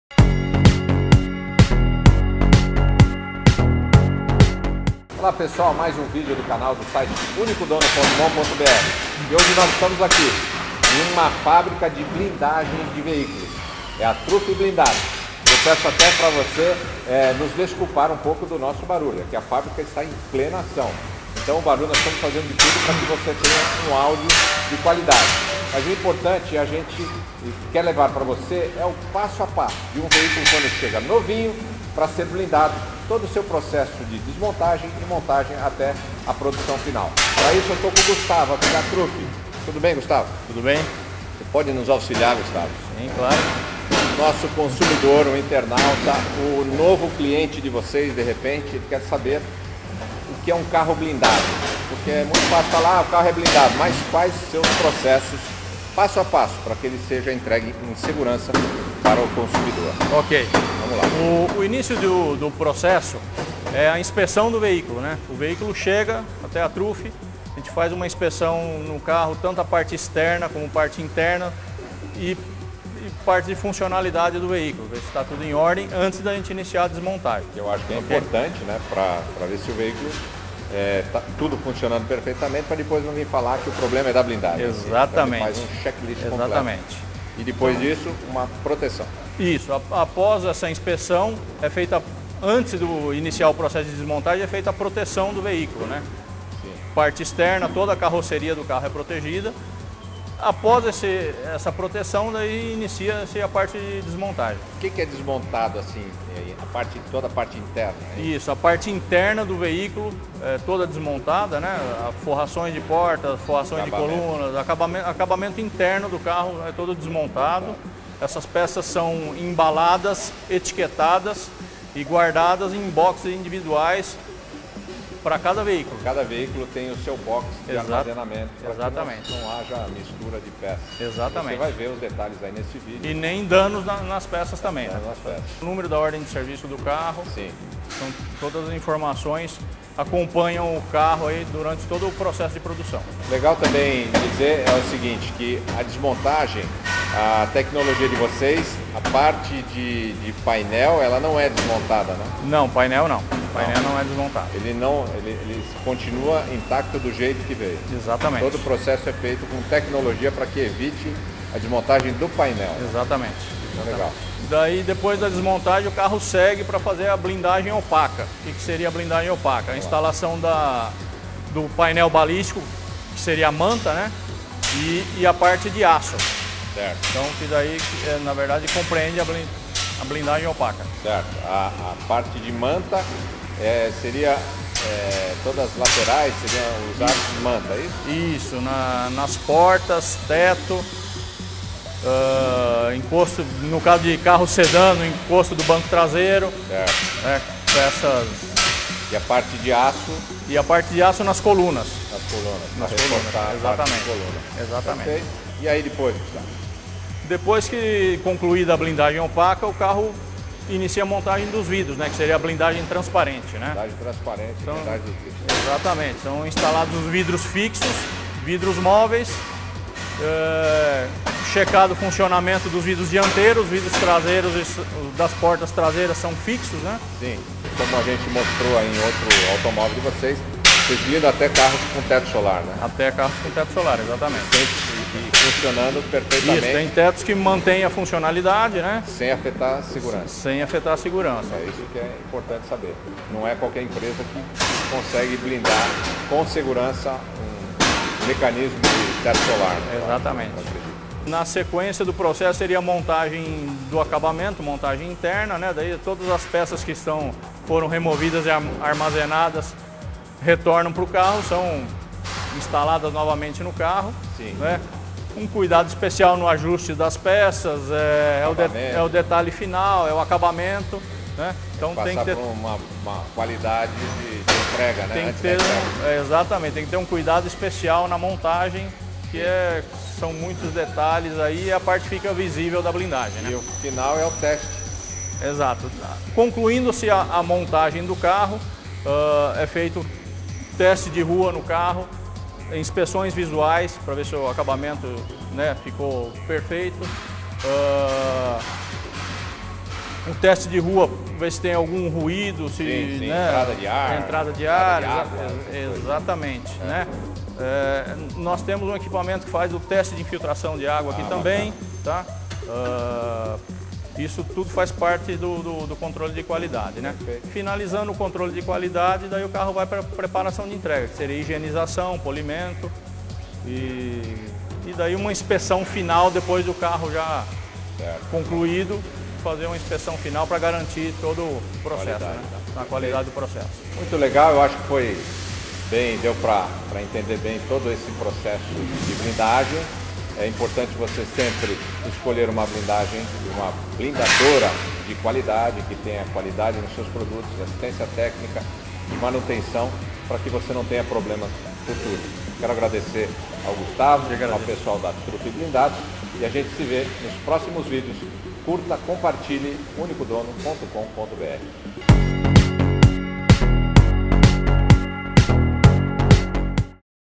Blindagem de Veículos - Como é Feito ?
Qual a melhor blindagem de veículos ?

Assista neste vídeo as etapas de como é feito uma blindagem em veículos.
Todo o processo foi relatado neste vídeo do , em entrevista com o pessoal da Blindadora Truffi Blindagens.

Com Rogerio Galvão

Link deste vídeo Tabela Fipe - consulte o preço de seu carro usado no Link abaixo